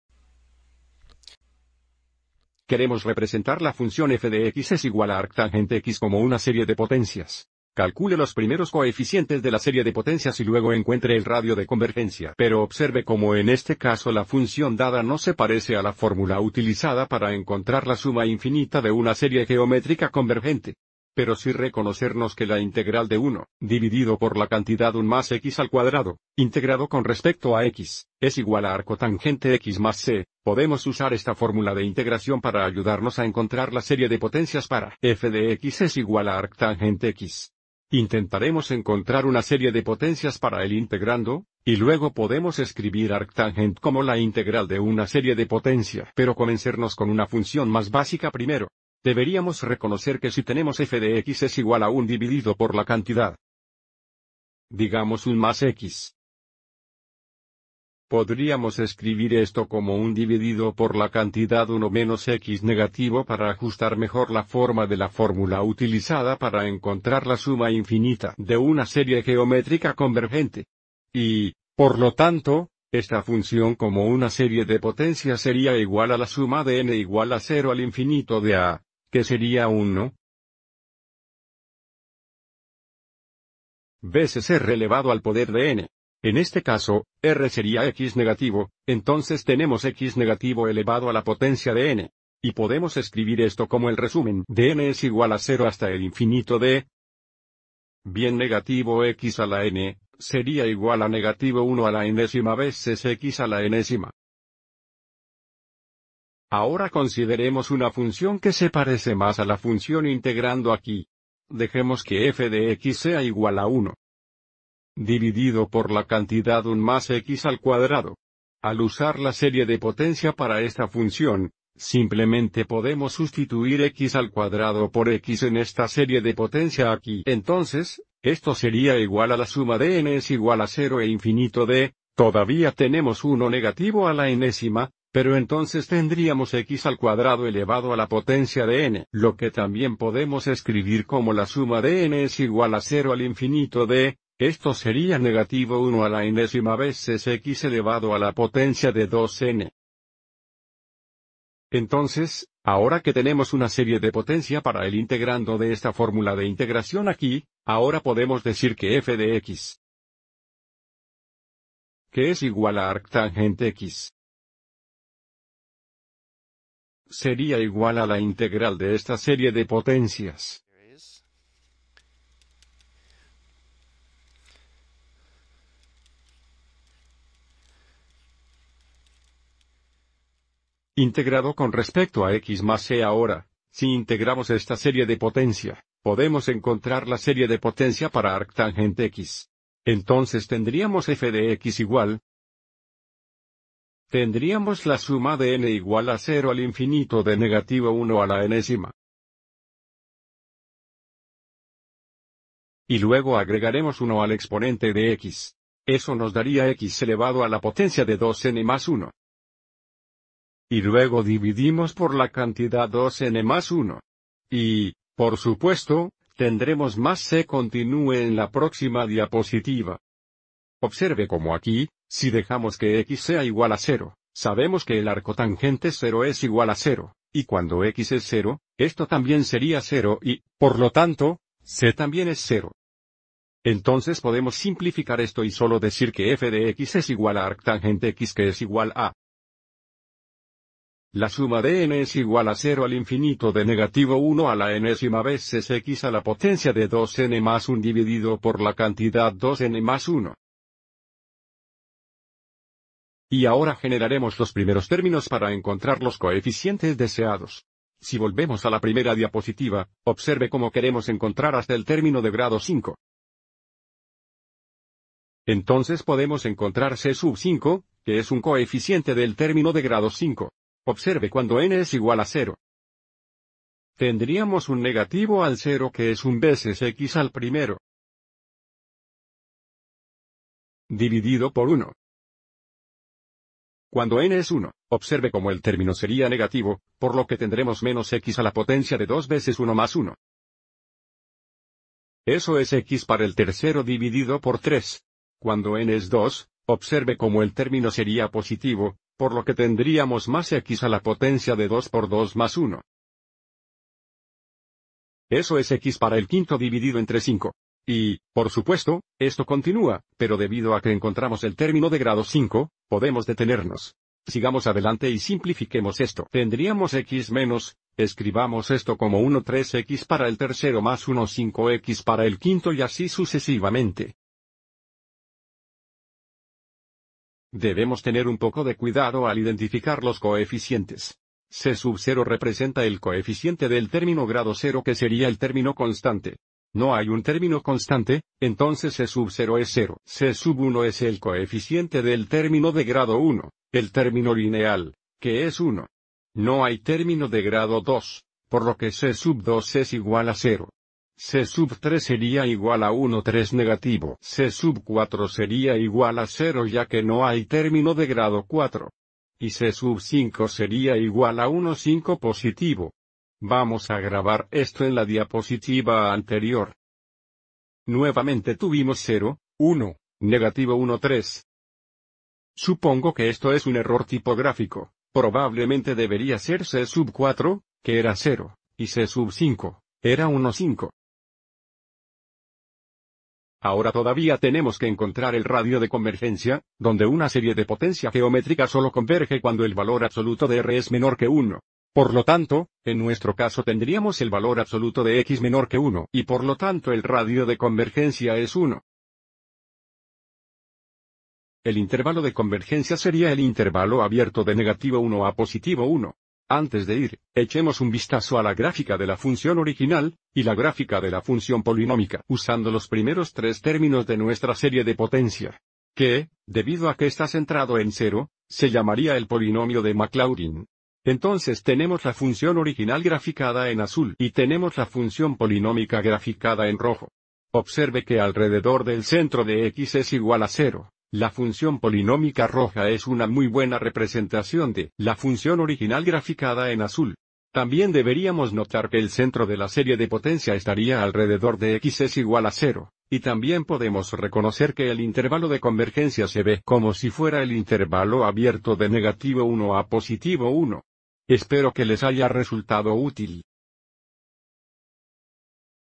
Ej: Encuentre una serie de potencia para representar un arctan (x) usando la integración
Este video explica cómo determinar una serie de potencias para representar f (x) = arctan (x) Sitio